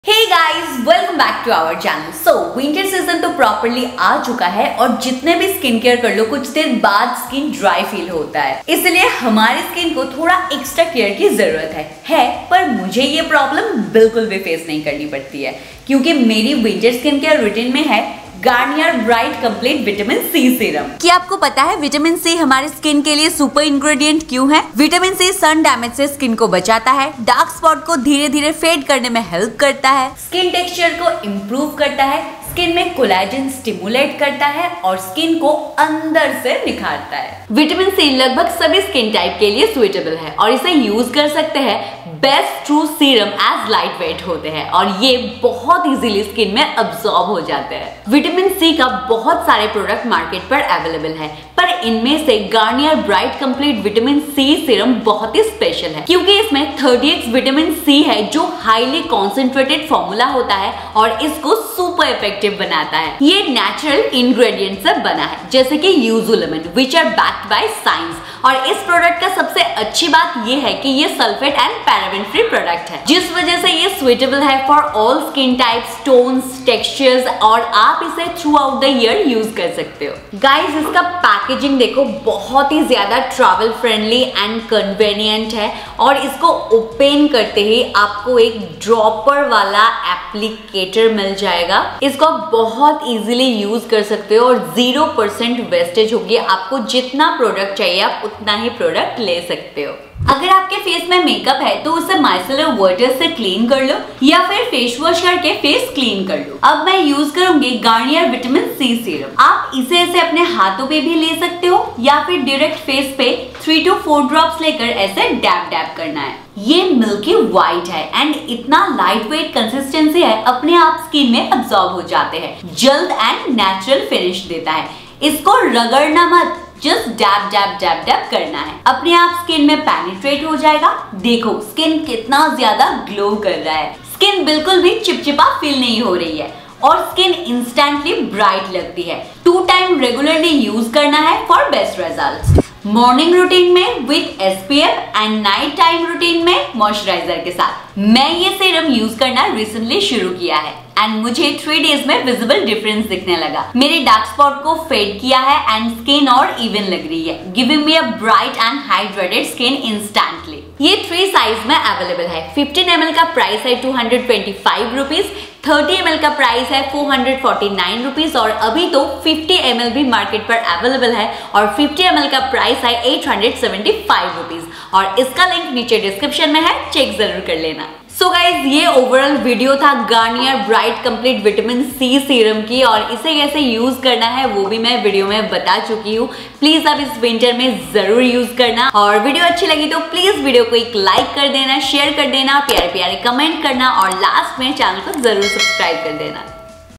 Get Super Glowing Smooth and Clear Skin at Home | Best Uses of VITAMIN C Serum
In today’s video, I’m going to talk about the key to achieving flawless, healthy-looking skin with ease! Yes, you read it right, you can now achieve the skin of your dreams by including the goodness of a vitamin C serum in your skincare routine regularly. So, without any further ado, let’s understand more about the benefits of vitamin C serum.
First things first, what is a serum? Serum is a skincare product that is highly concentrated, very lightweight and blends in the skin effortlessly to help you get maximum benefits. Now, if you are a skincare newbie, you need to learn about which ingredient will work the best for you to banish all skin concerns effortlessly, especially as the climate changes. My favourite ingredient when looking for a serum has to be vitamin C as it is known for its antioxidant properties, which means it helps reduce dark spots while brightening the skin and acts as an anti-ageing solution as well. Not sure how to incorporate vitamin C benefits into your skincare routine? I highly recommend getting your hands on the Garnier Vitamin C serum for the face to get visible results in no time. With 30X vitamin C, it is made with natural ingredients like Yuzu lemons and backed by science to fade dark spots and get an even skin tone instantly. What’s more, you can use this serum for all skin types. That’s not it. The Garnier vitamin C serum is paraben-free and sulphate-free, making it one of the best vitamin C serums in town.
Speaking of the packaging, the vitamin C serum comes in a compact, attractive bottle with a dropper that makes the application super easy. The serum is made of a milky white, not-too-thin, not-too-thick, lightweight, non-sticky texture that absorbs into the skin quickly. All you need to do is use the dropper and pour three to four drops of serum on a perfectly cleansed face. Then, tap and massage gently into the skin and that’s  it. Once done, don’t skip applying a hydrating moisturiser and SPF to protect your skin from environmental aggressors. For best results, use the vitamin C serum twice a day.
Get more vitamin C at a lesser price. Available in three sizes – buy the Garnier vitamin C serum 15 ml for Rs. 225, 30 ml for Rs.449 and 50 ml for Rs. 875 only.